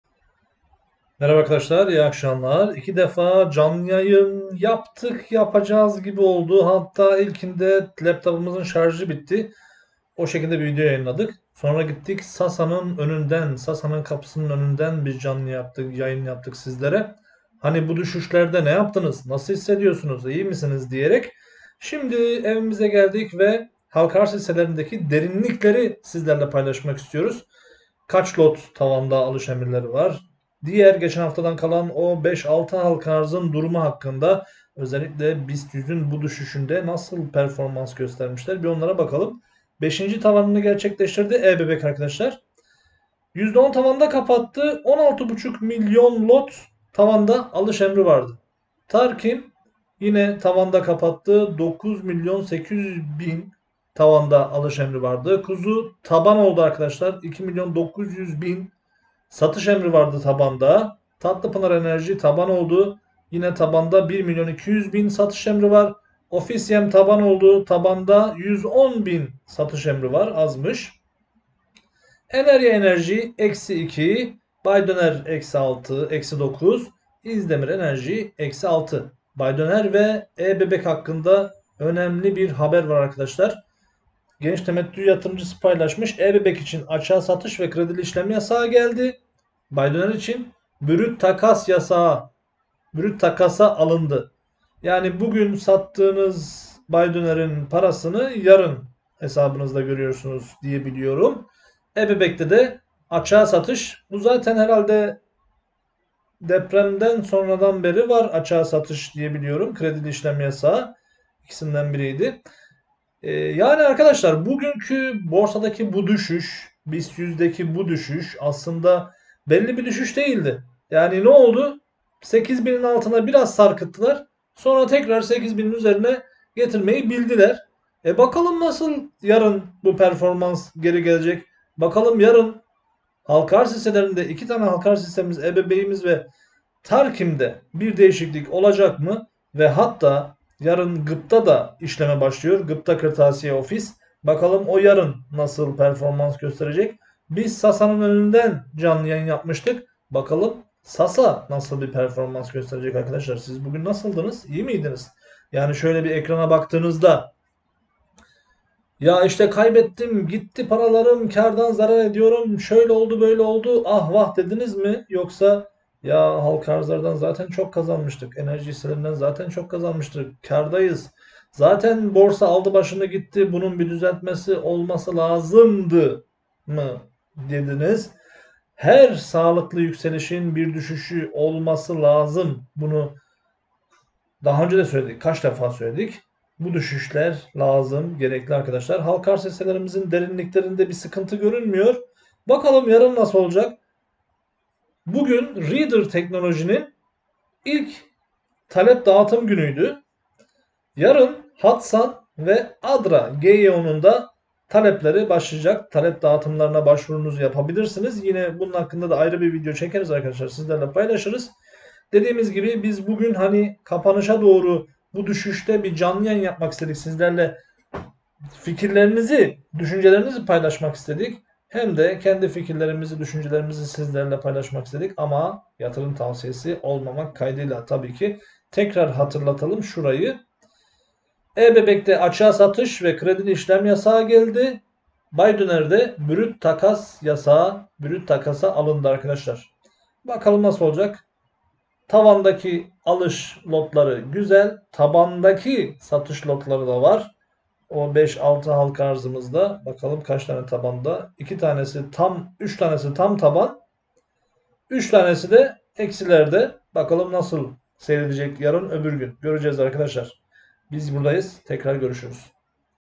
Halka Arz Hisselerinde Tabandaki ve Tavandaki Emirler
Halka arz hisselerinde kapanış verileri geldi ve Ebebek ve baydöner işlem yasağı geldi.